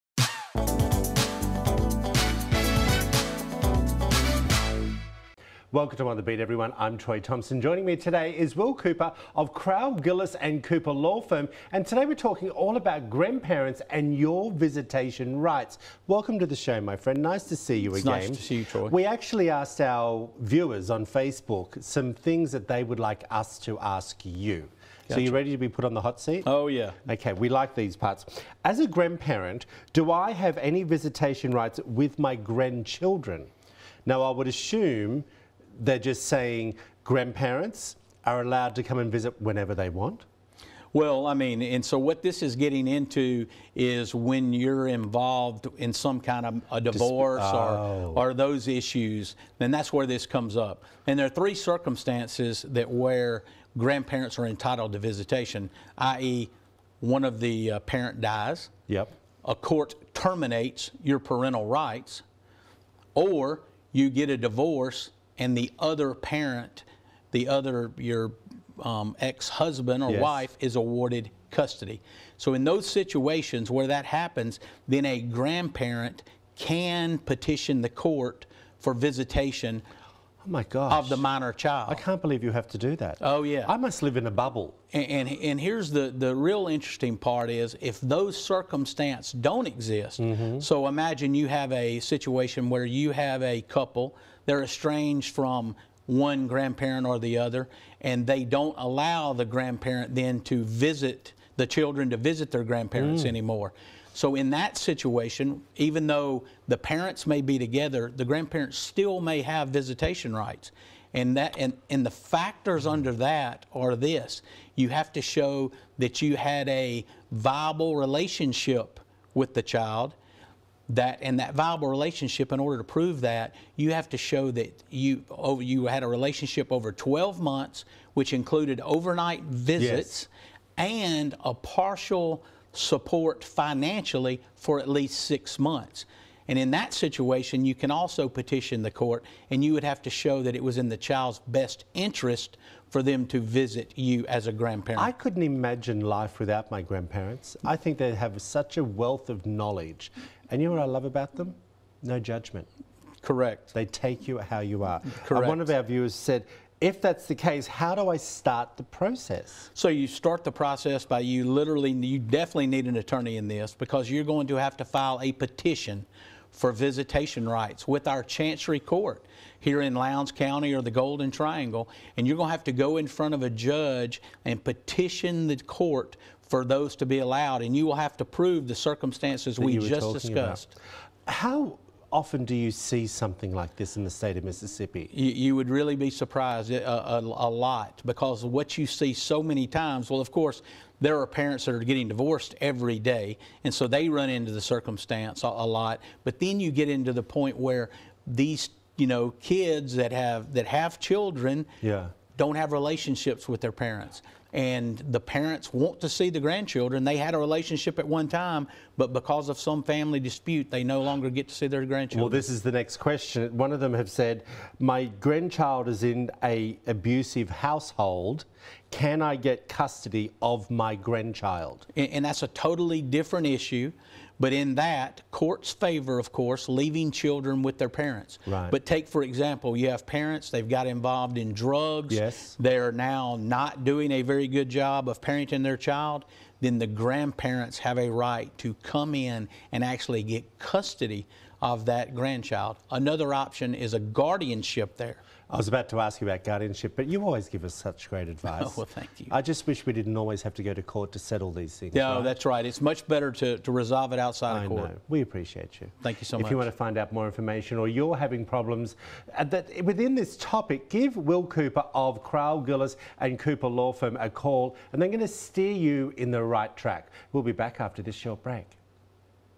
Grandparent Visitation Rights with Will Cooper
When certain family issues arise, one might need to know proper visitation rights for grandparents. Will Cooper and Troy Thompson go into detail about the various visitation rights for grandparents during these situations. Watch now to learn how Crowell Gillis & Cooper, PLLC can help.

Information

Crowell Gillis & Cooper, PLLC

Located in Columbus, MS